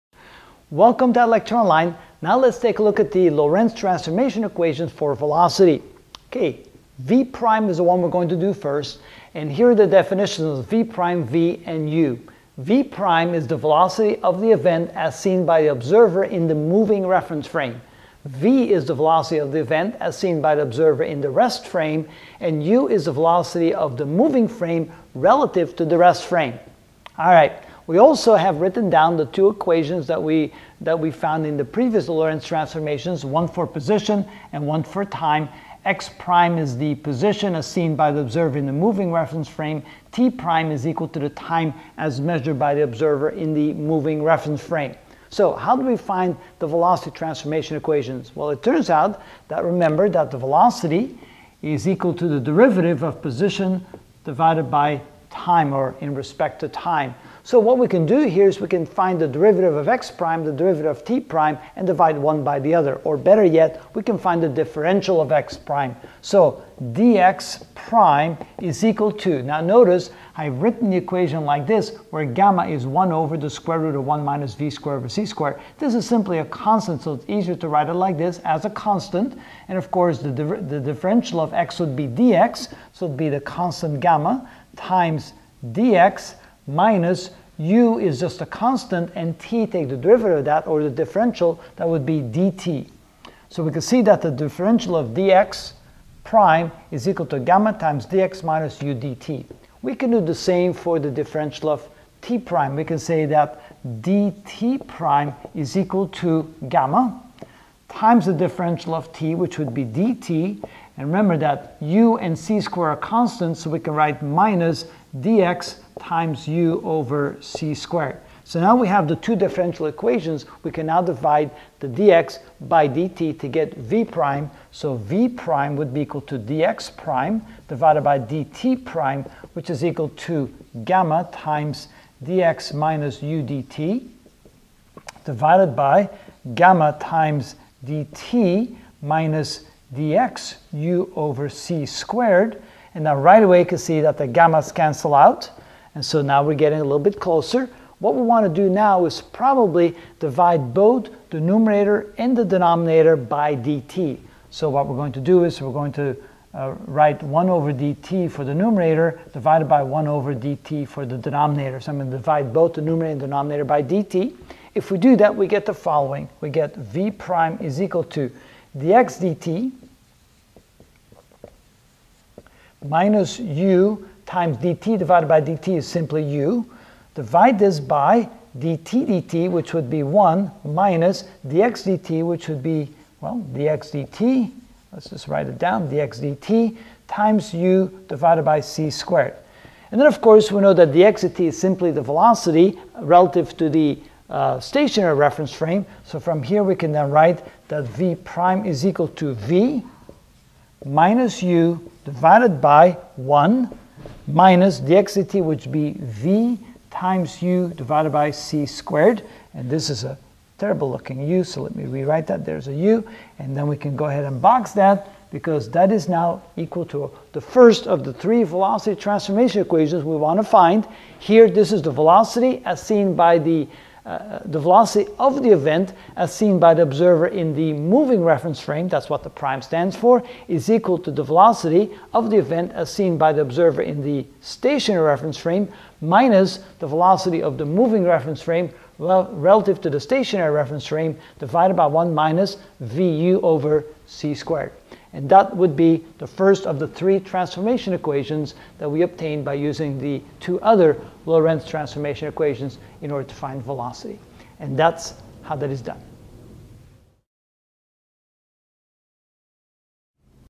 Physics 62 Special Relativity (23 of 43) The Lorentz Transformation Equations: Velocity (v')
In this video I will find Lorentz transformation equation in regards to velocity (v').